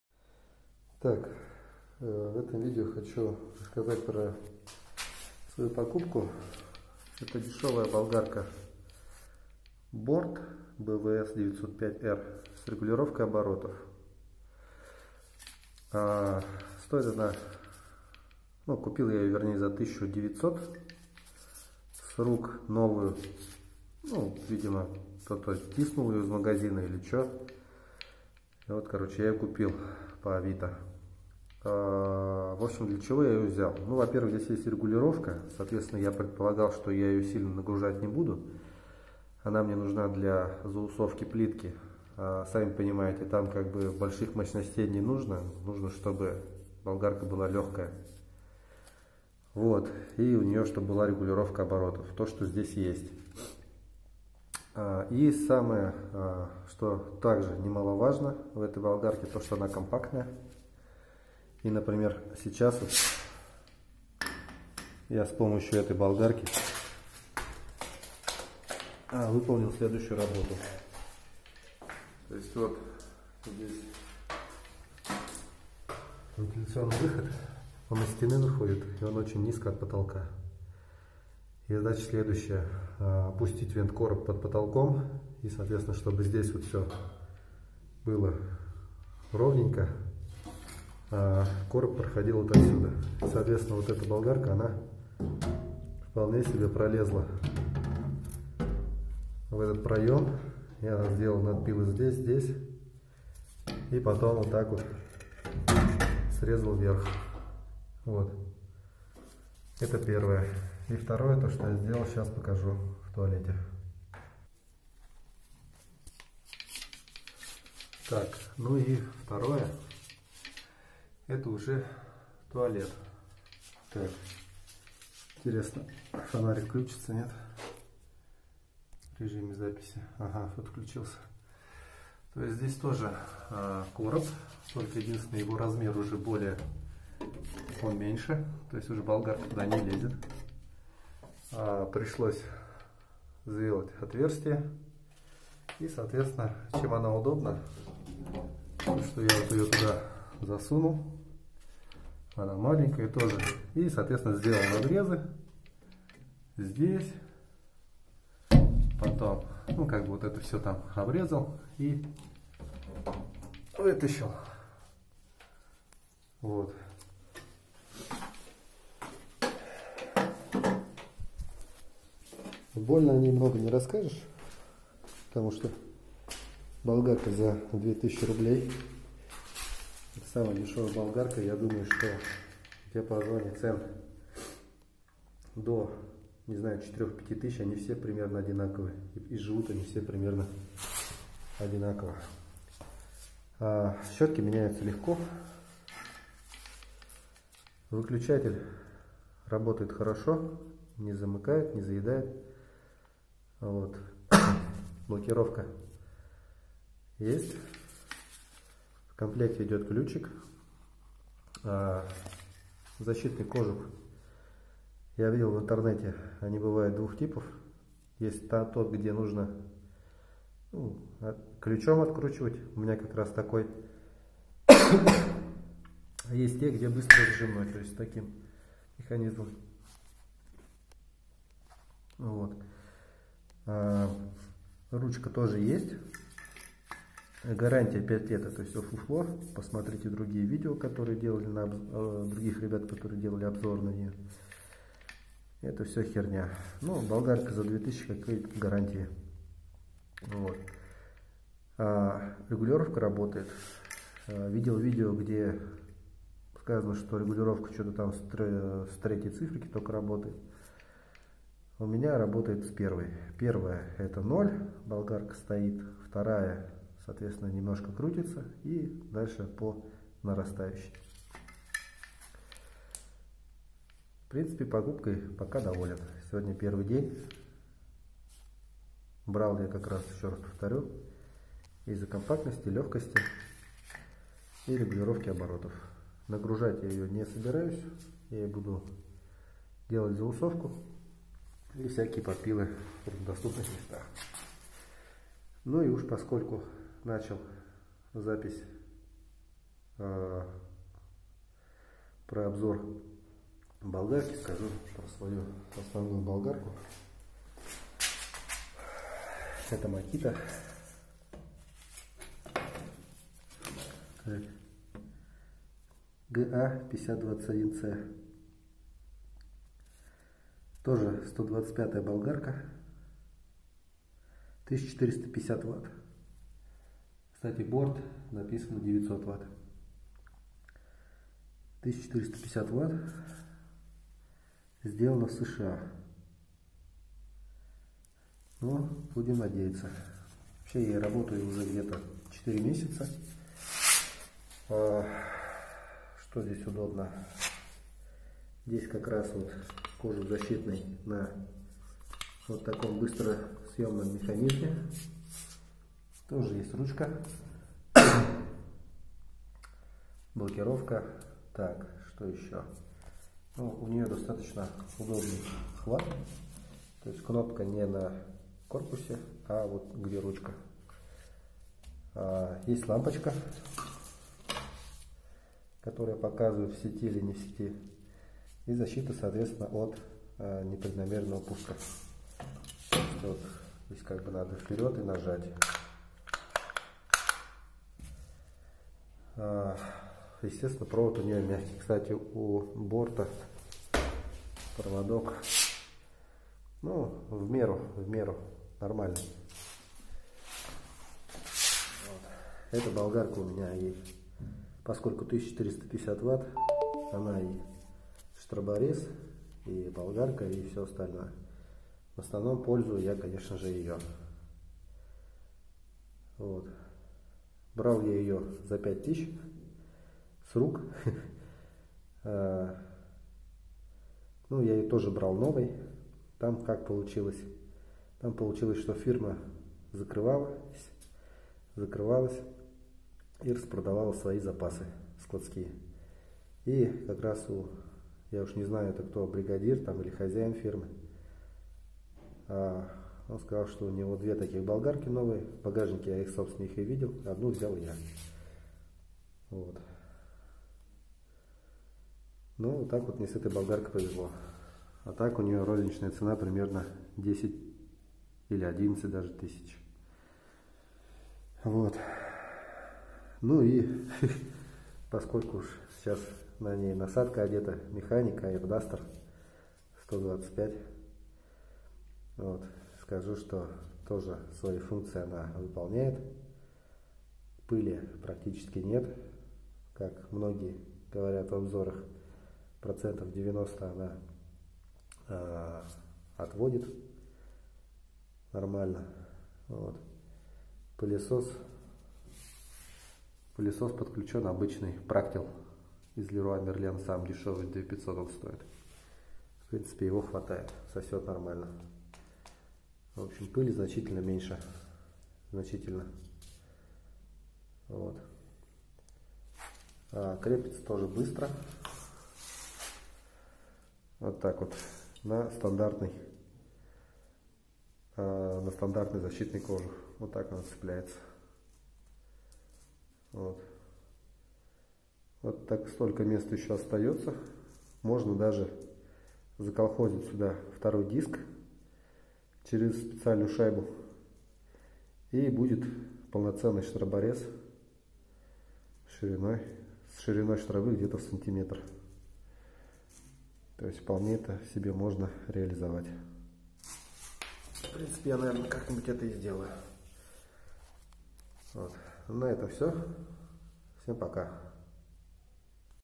Обзор Bort bws 905 r, Makita GA5021c, Airduster 125
март 2021. спустя 2 года всё работает. Борт - стал барахлить регулятор оборотов.